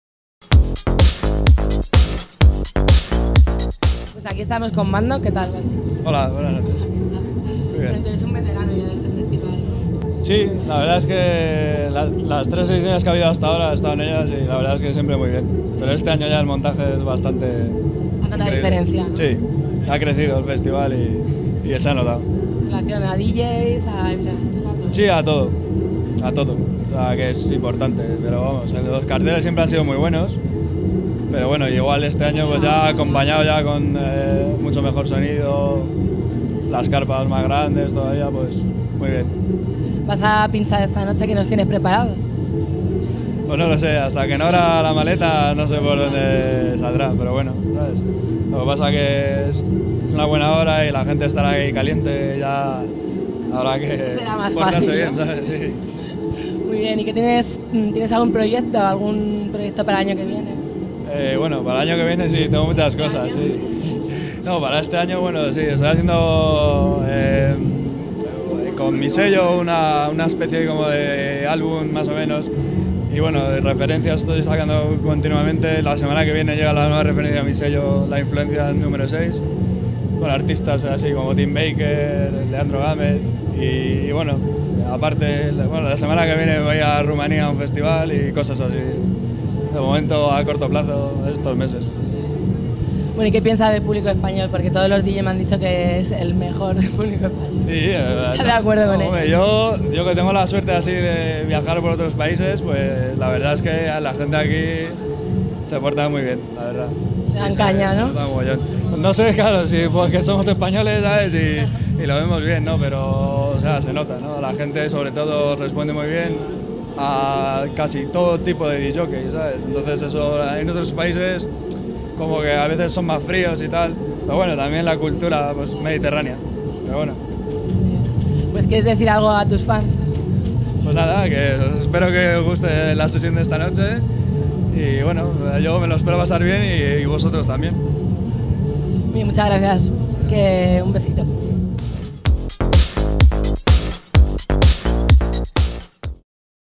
Electrosonic 2007 - entrevista a Dj Bando
Entrevista a Dj Bando a raiz de su actuación en Electrosonic 2007